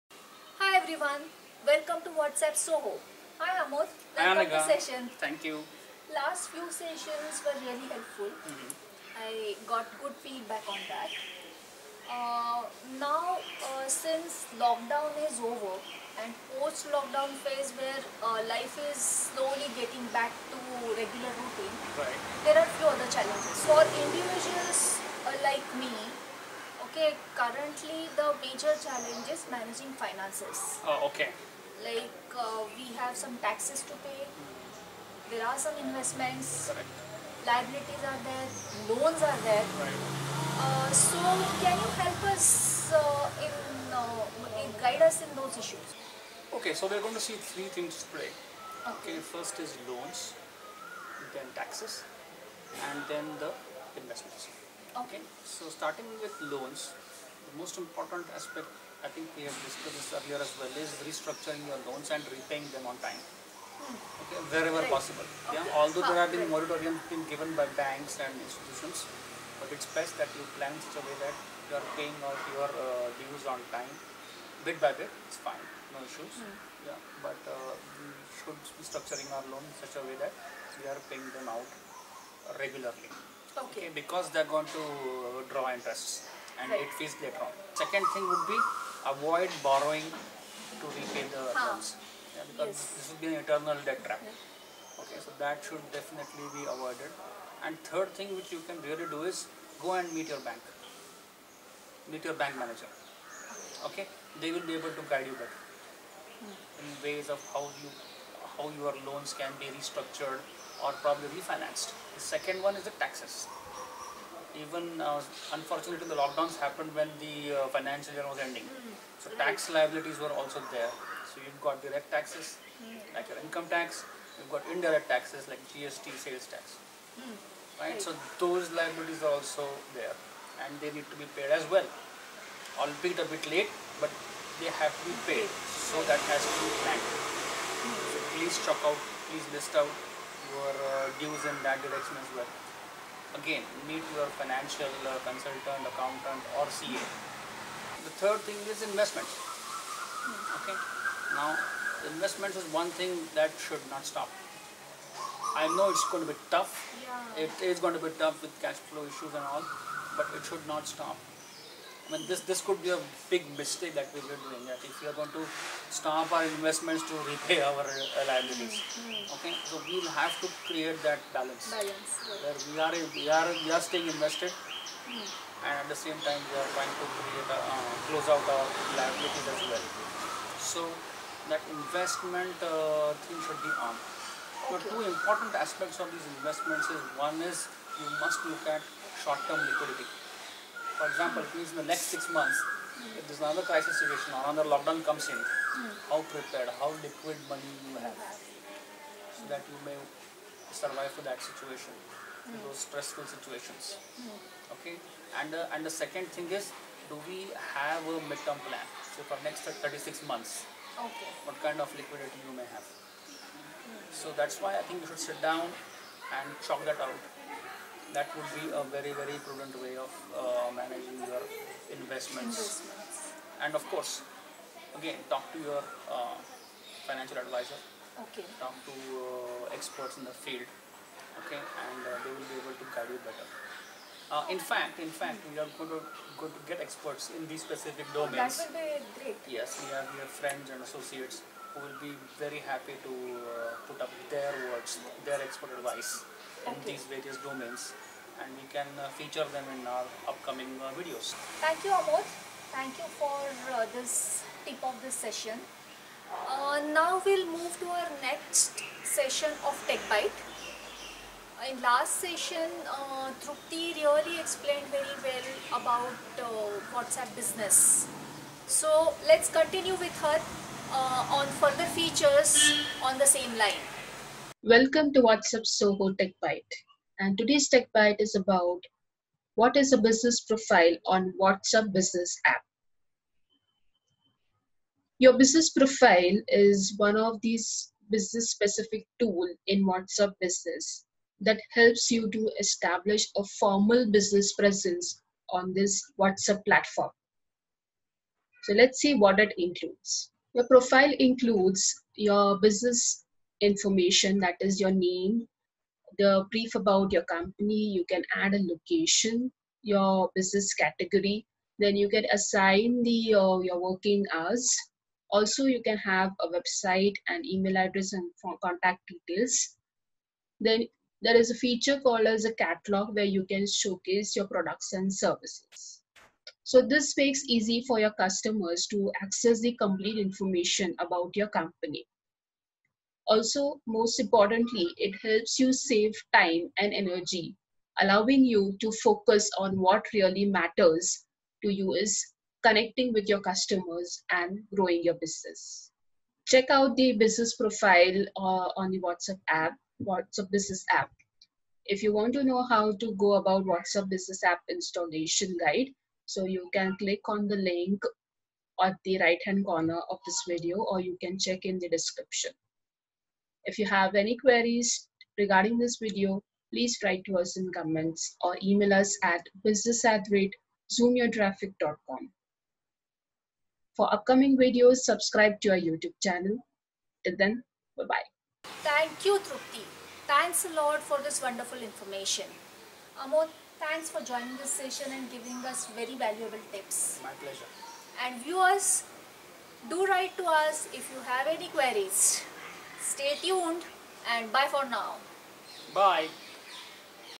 WhatsUp SOHO - Managing Loans, Taxes and Investments As Unlocks Begin....
WhatsUpSOHO - As markets open and lockdowns are relaxed, we all need to get things going especially on the economy. Our own personal economy needs addressed through Loans, Taxes and Investments. How prepared are we to handle them effectively?

Powered by:
ZoomYourTraffic Web Solutions